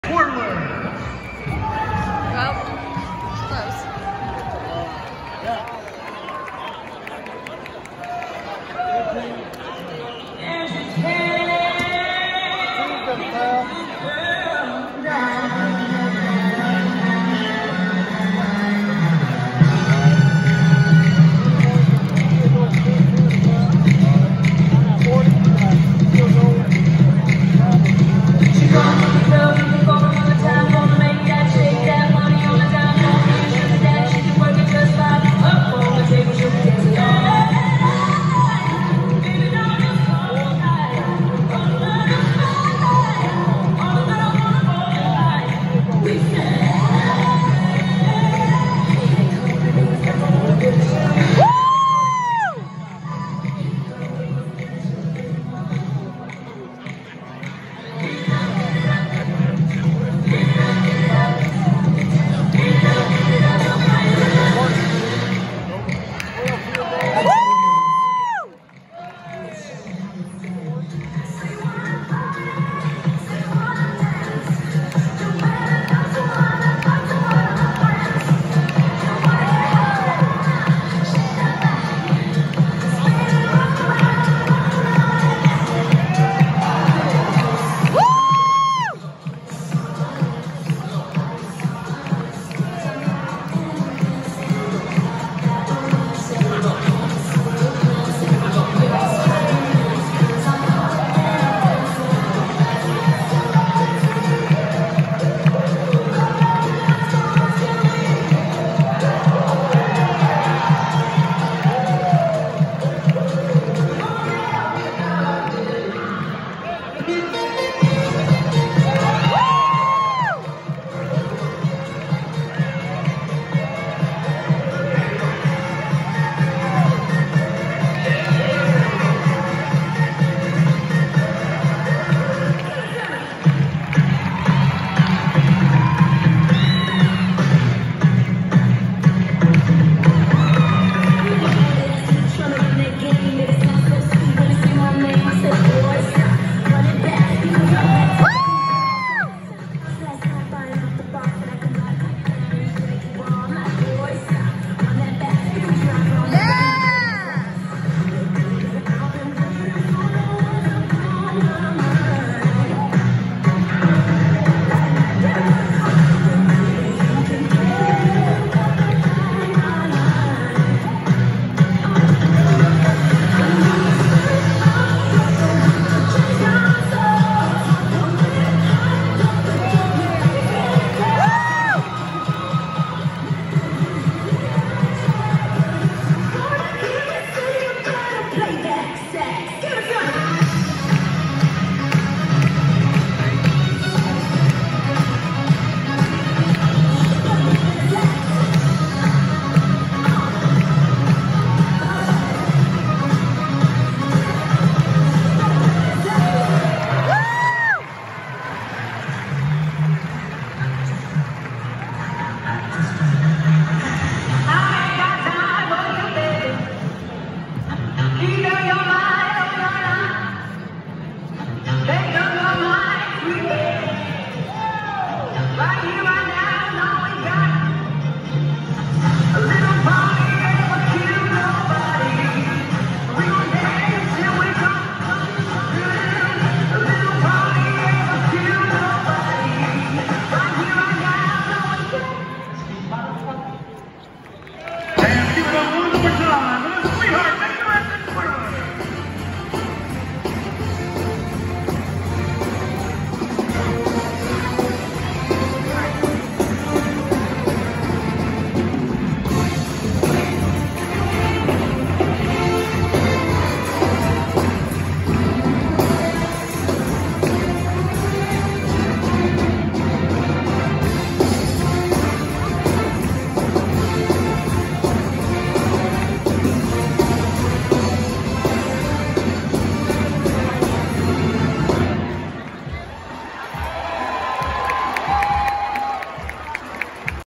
UK Sweetheart Majorettes and Feature Twirlers - 1/16 Women’s Basketball Game Halftime
UK Sweetheart Majorettes and Feature Twirlers - 1/16 Women’s Basketball game halftime performance against Texas A&M